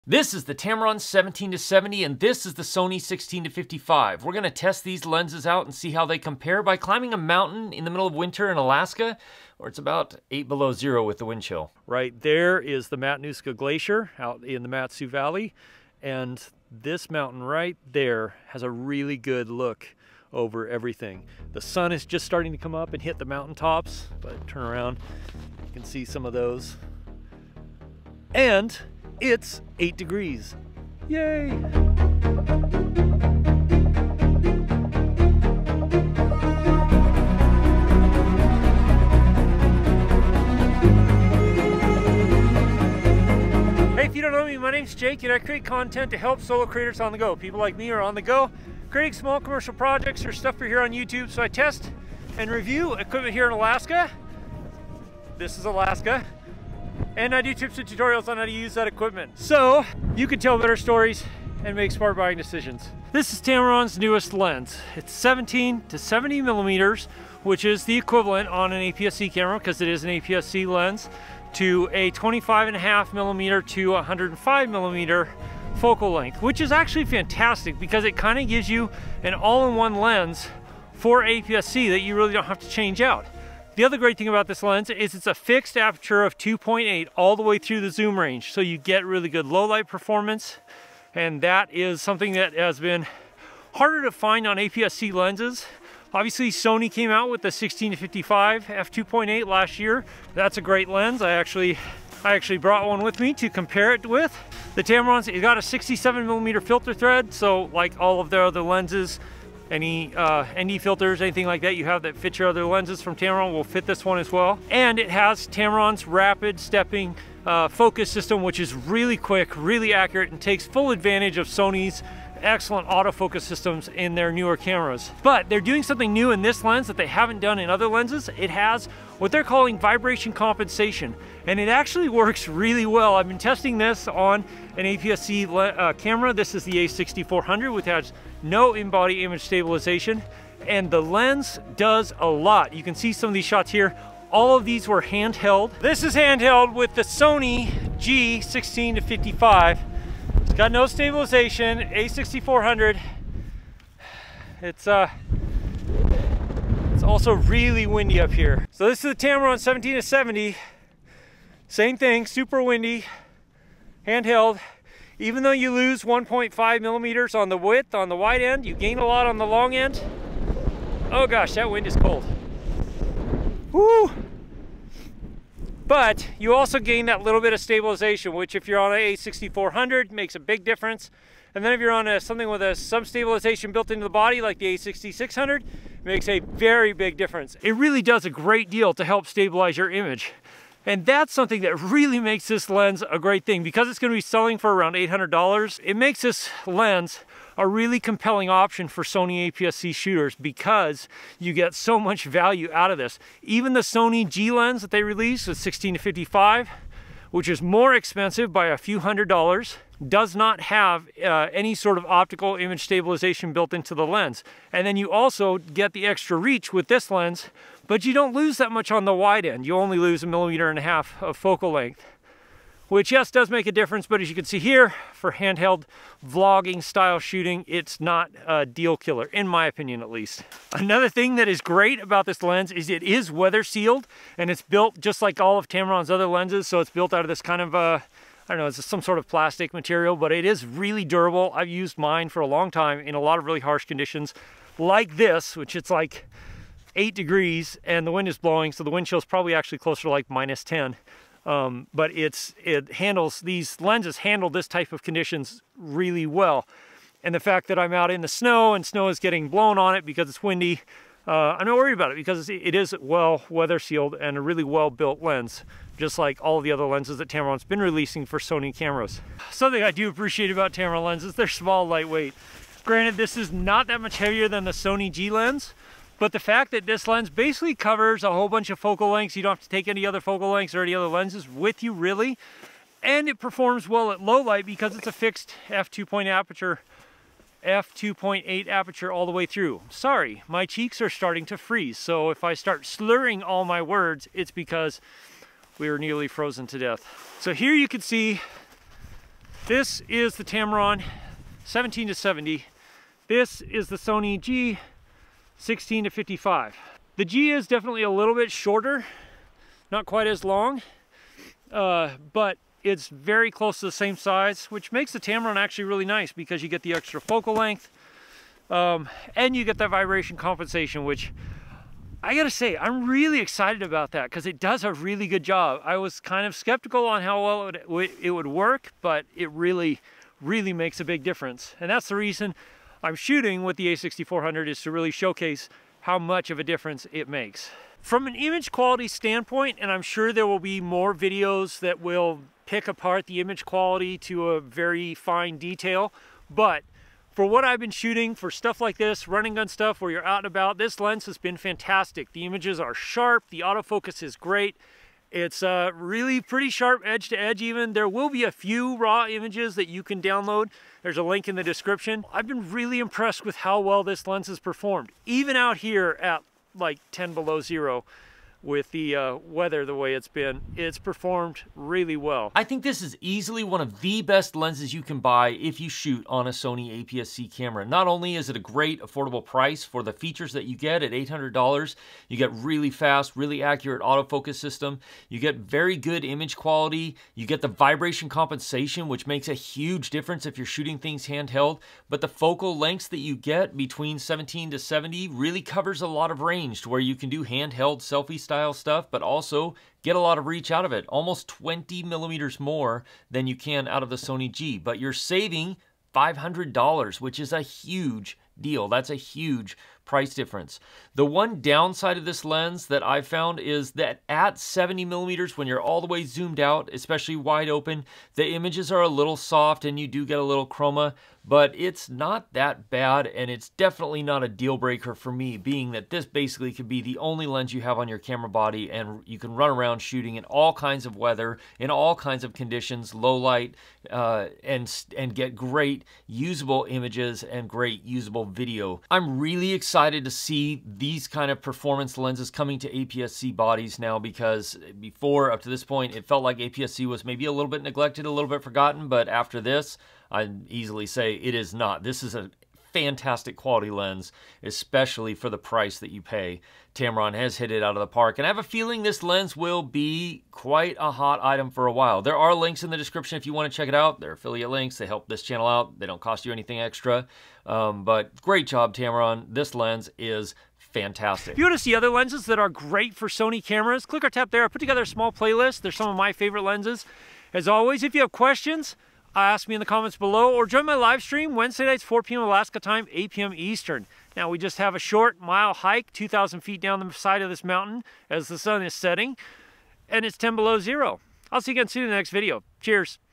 Tamron 17-70 Sony The Best Lens for APSC in 2021
Tamron is releasing the 17-70 For Sony APSC cameras like the a6400 a6600 and a6100. After using this lens for the last month I feel I can easily say this is the best lens you could buy for a Sony APSC camera. The Tamron 17-70 is light weight, small, versatile and delivers fantastic image quality over almost the entire zoom range. The 17-70 f2.8 means it also performs well in low light for astrophotography, and since it has image stabilization built into the lens it can make an excellent lens for handheld filming and photography work! To test the tamron 17-70 I took it with me on a few trips in Alaska and even compared and tested it against the Sony 16-55 f2.8 G lens. Over all The Tamron 17-70 f2.8 is a fantastic lens and easily one of the best For Sony APSC cameras.